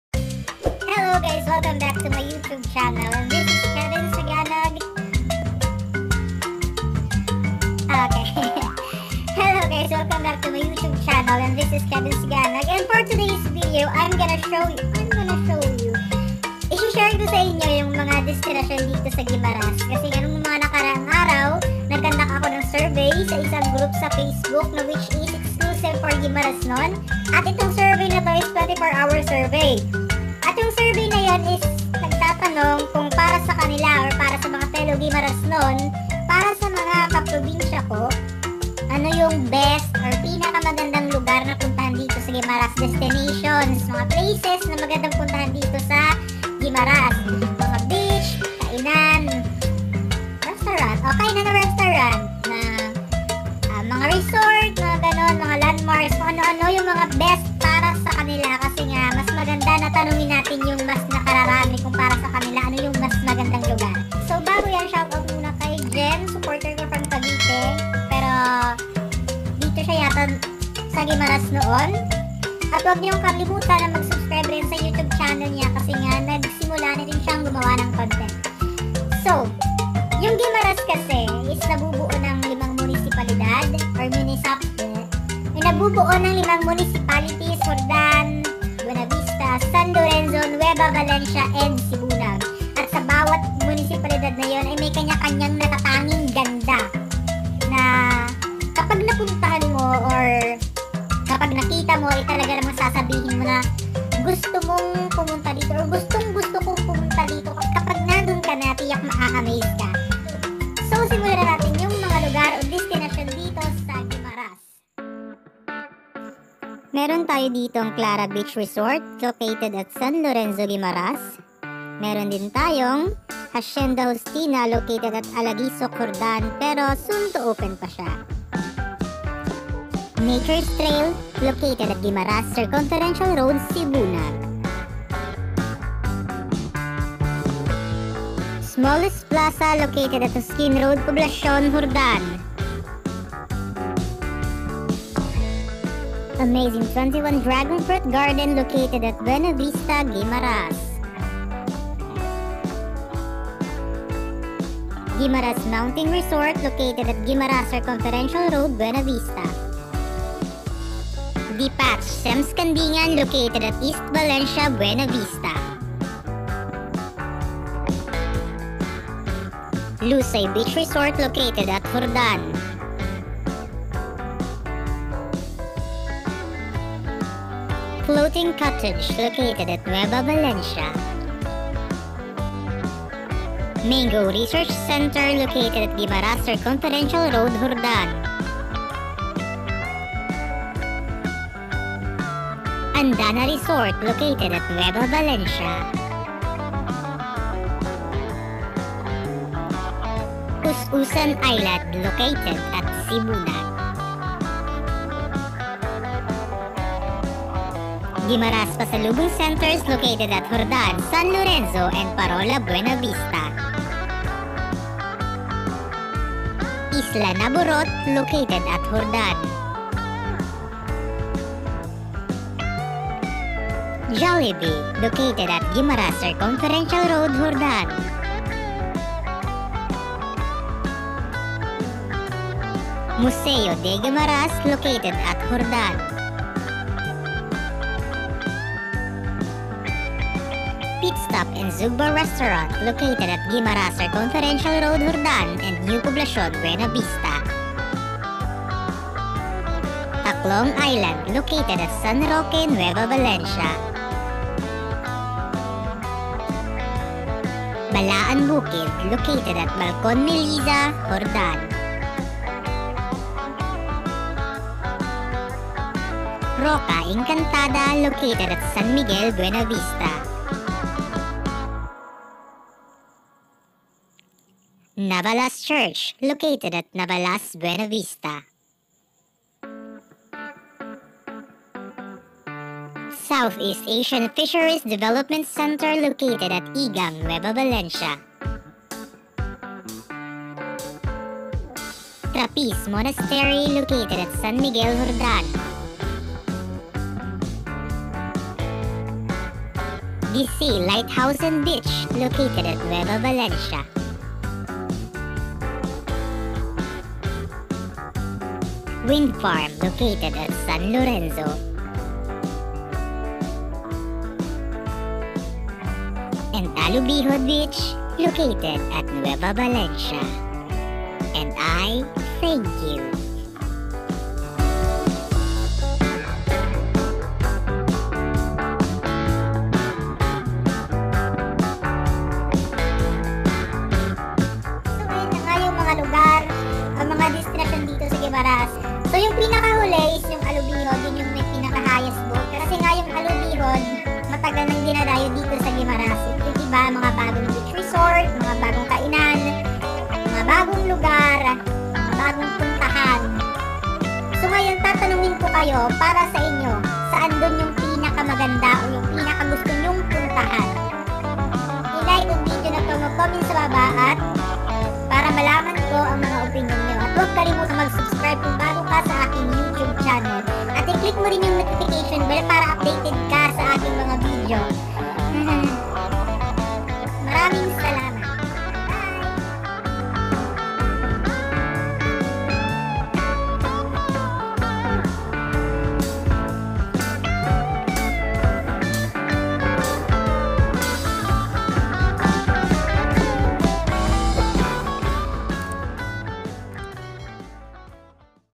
GUIMARAS Top Destinations/Places
I would like to thank the "TAGA GUIMARAS AKO" Facebook Group for helping on the survey.

So ito na yung result ng survey ko. Please watch and let me know your opinion on the comment section.

"This channel and I do not claim any right over any of the images used in this video.